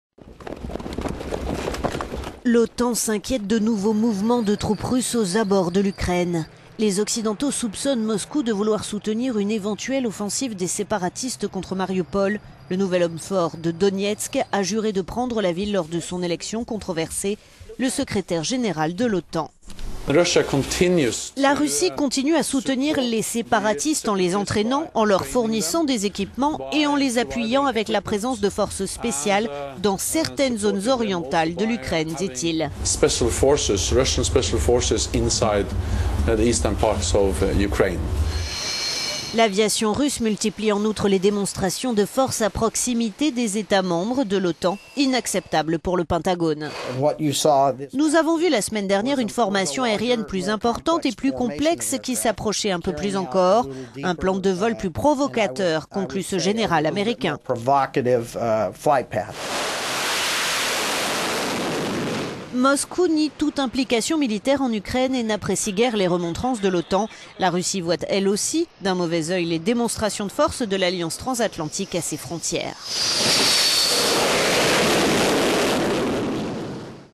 Des mouvements de troupes russes près de l'Ukraine inquiètent l'OTAN
Les Occidentaux soupçonnent Moscou de vouloir soutenir une éventuelle offensive des séparatistes contre Marioupol. Le nouvel homme fort de Donestk, Alexander Zakharchenko, a juré de prendre la ville lors de son élection controversée.

"La Russie continue à soutenir les séparatistes en les entraînant, en leur fournissant des équipements, et en les appuyant avec la présence de forces spéciales dans certaines zones orientales de l'Ukraine", a déclaré hier le secrétaire général de l'OTAN, Jens Stolt…